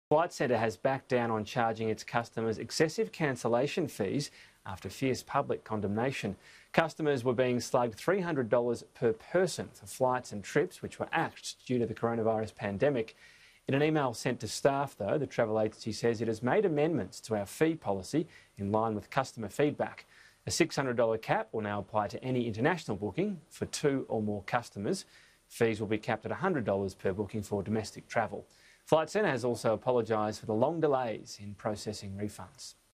Flight Centre backs down on cancellation fees
Flight centre has backed down on charging its customers excessive cancellation fees after receiving fierce public condemnation.

Customers were facing $300 cancellation fee per person for flights and trips axed due to the coronavirus pandemic.

A $600 cap will now apply to any international booking for two or more customers and a $100 cap per booking for domestic travel.